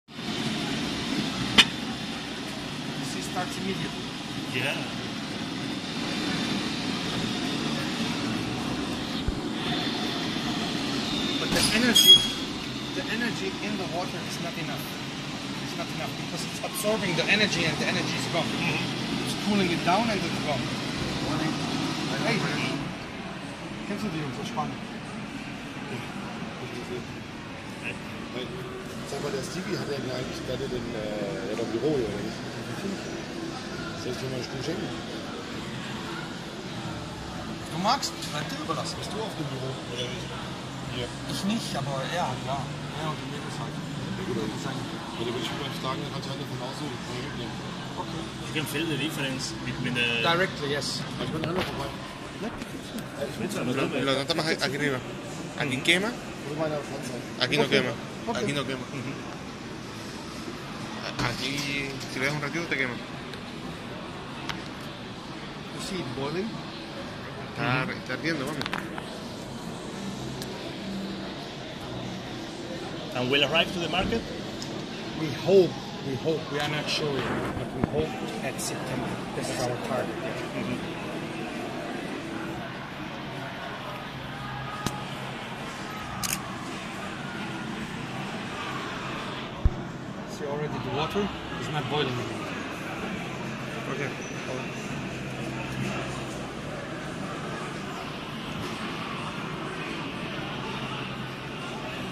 Sistema de refrigeración pasivo de Raijintek
Raijintek mostró a puerta cerrada durante la Computex un prototipo del que será su sistema de refrigeración por agua pasivo. No incorpora una bomba de agua ni ventiladores.